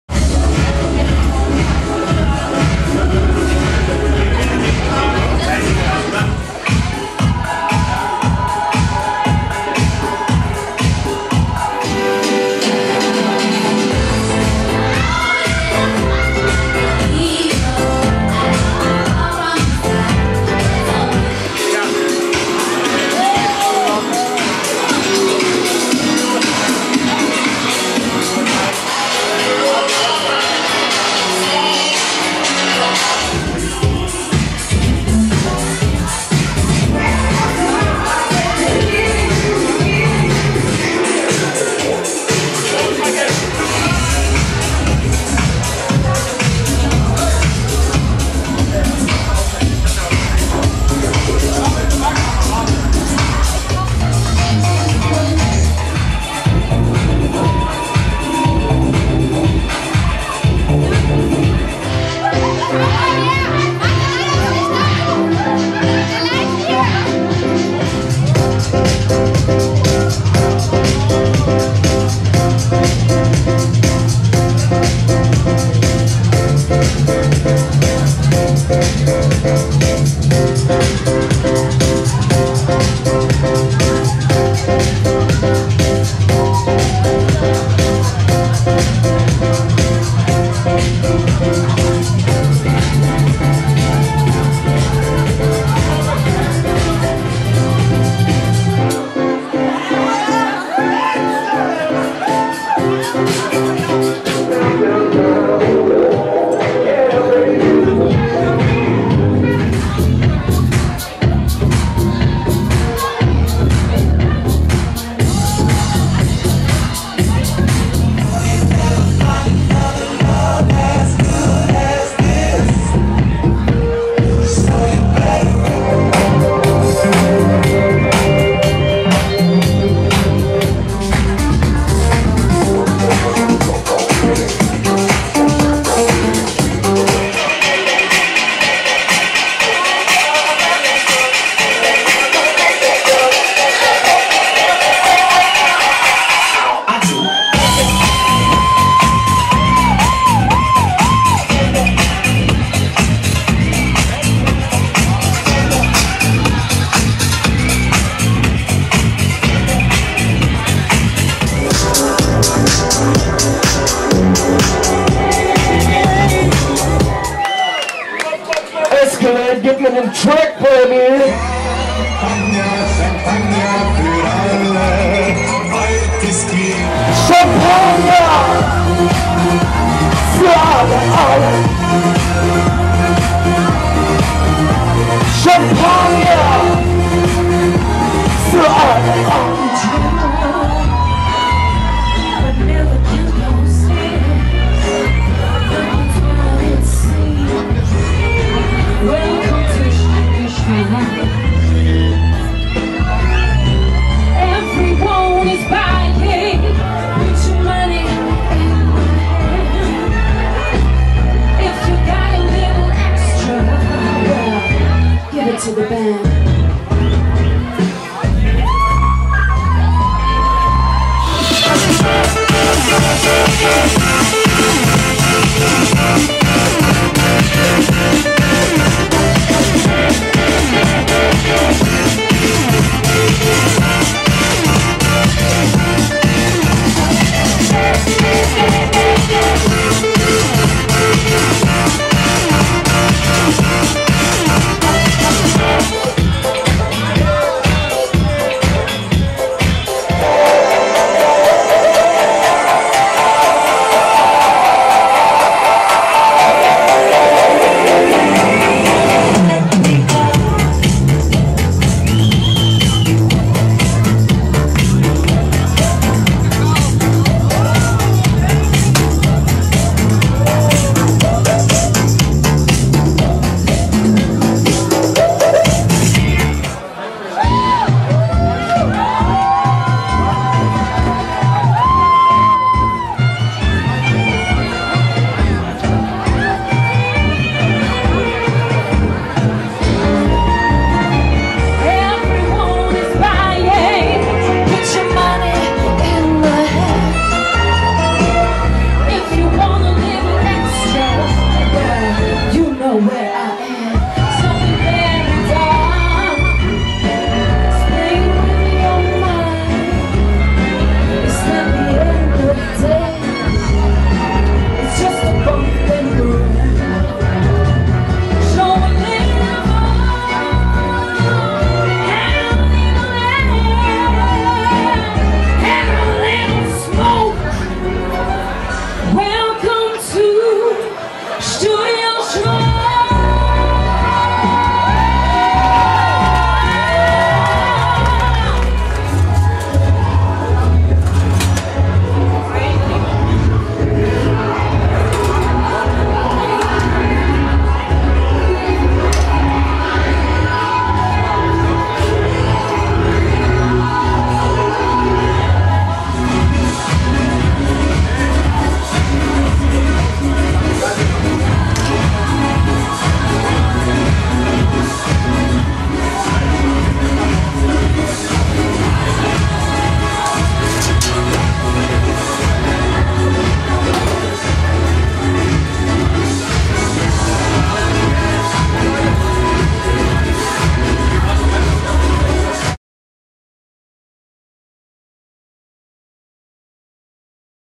Opening Club "Studio Schwarz" im P1 mit großer Opening-Party vom 17.01.-19.01.2013
Opening Club "Studio Schwarz" im P1 mit großer Opening-Party vom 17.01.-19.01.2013. Anfang 2013 erfindet sich das P1 mal wieder neu. Nur etwas mehr als zwei Jahre nach dem letzten großen Umbau, nun also schon wieder etwas Neues, zumindest im Stüberl, die Lounge neben dem Clubbereich. Dort entstand ein völlig neues Club-in-Club Konzept - das ehemalige Stüberl verwandelt sich zum "Studio Schwarz".

Neben dem neuen DJ Pult gibt es als zentralen Bestandteil des neuen Studio Schwarz Konzepts zum ersten Mal in der P1 Geschichte eine Bühne, auf der Künstler aus allen Bereichen auftreten werden.

Die Decke des 170 qm großen Studios erhält 20 LED-Videopanels, und auch an den Wänden hängen sechs ein mal zwei Meter große Screens, die von Videokünstlern bespielt werden.